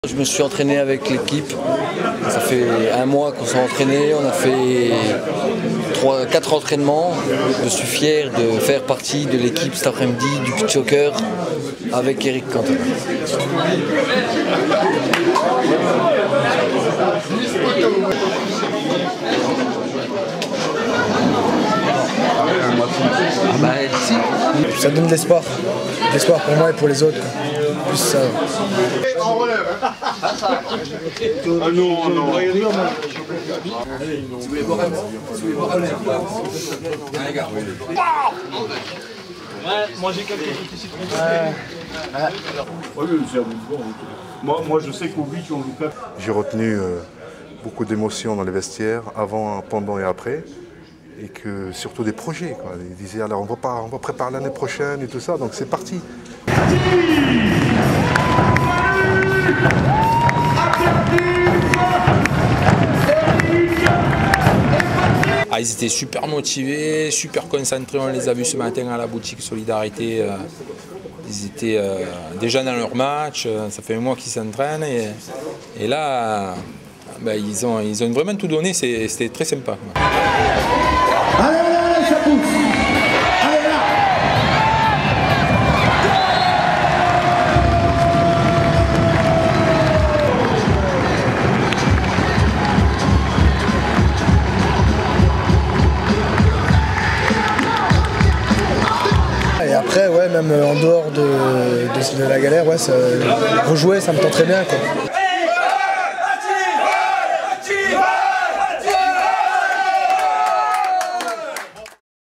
Rock'nBeachSoccer 2009 à Metz : les Amis et Parrains de la Fondation jouent avec des sans-domicile.
Dimanche 1er mars 2009, Patrick Bosso, Eric Cantona et Abdelatif Benazzi ont joué avec treize accueillis de la Boutique Solidarité de Metz et de la Pension de famille de Woippy lors de la 2è édition de la Beach Soccer Solidarity Cup organisée par Joël Cantona.